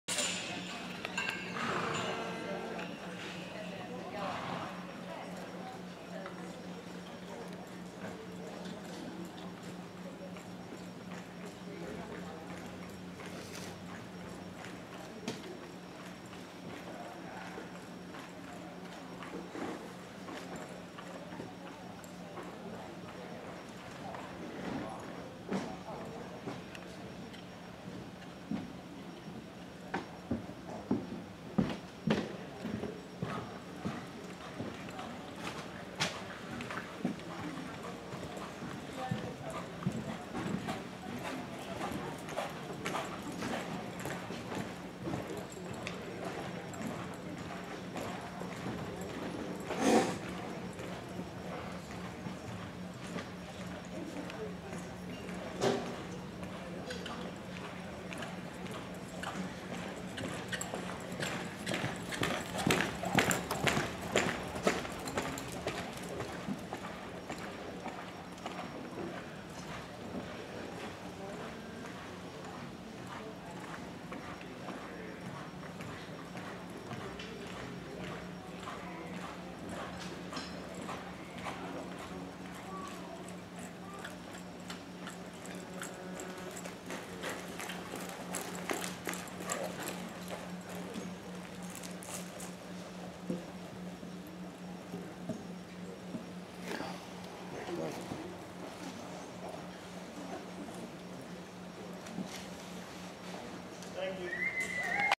CWAQHA show Open Ranch riding with Jimmy Brown & Alluminati
Jimmy Brown showing 4 yr. old red roan stallion Alluminati in Open Ranch Riding at the Central Washington QHA show in Moses Lake, /WA.  Visit our FB Windstar Ranch page to see more pictures and videos of our outstanding horses.